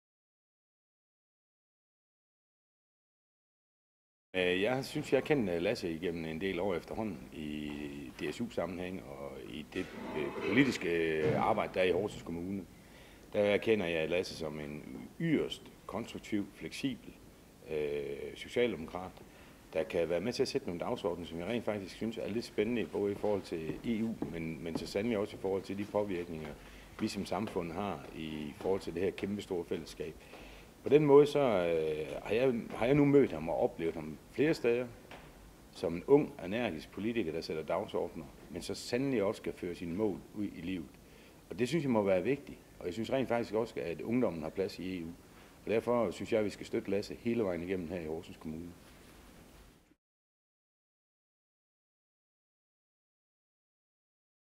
Peter Sørensen Stem på Lasse
Borgmester Peter Sørensne, Horsens, opfordrer vælgerne til at stemme på Lasse Quvang Rasmussen den 25. maj